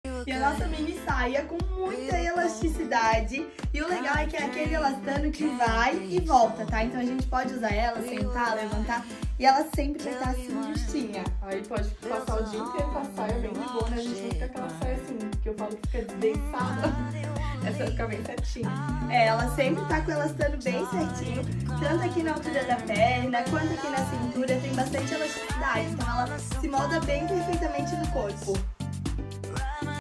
Mini Saia Jeans Hot Collor - Cosh Jeans
A mini saia jeans hot collor da Cosh é uma ótima opção de peça para se usar nos dias de verão e eventos casuais. A saia jeans é uma peça versátil que pode ser combinada com cropped, T-shirt, camisa ou até mesmo body’s. Quando se tem uma saia jeans da Cosh, no armário, já podemos considerar a metade do look feito. É a peça perfeita para compor os looks mais descolados e atemporais.